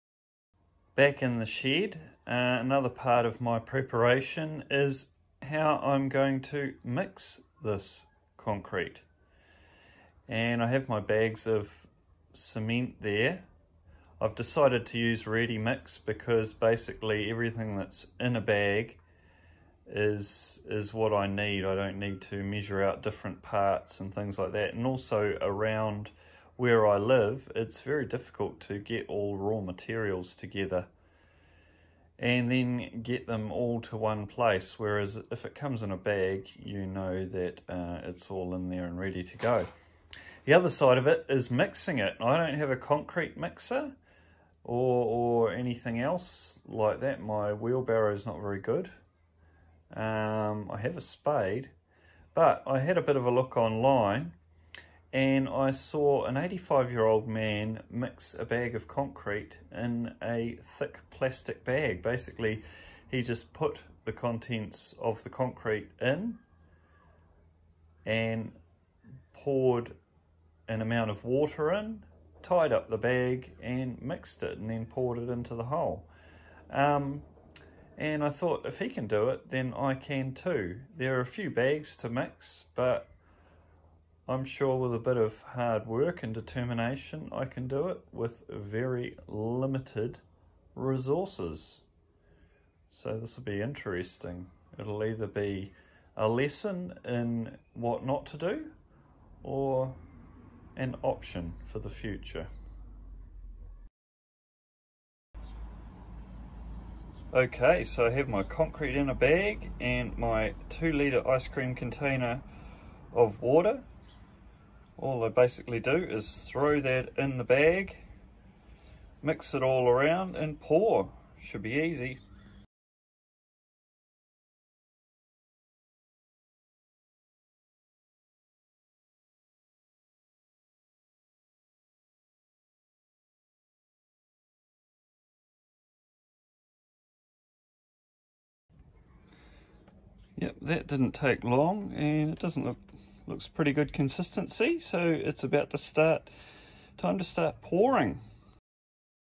Mixing Concrete in a Plastic Bag - Minutes to Learn and Make
Learn how to mix concrete in minutes with just a plastic bag. Adam doesn't have a concrete mixer so decides to try mixing cement in an old insulation bag destined for the landfill. Sounds easy and is easy. Interestingly Adam mentions 'bags of cement'. Cement is actually something that is mixed with aggregates and this is then mixed with water which then hardens into concrete. A ready-mix concrete bag will require just water to make the mixture for concrete.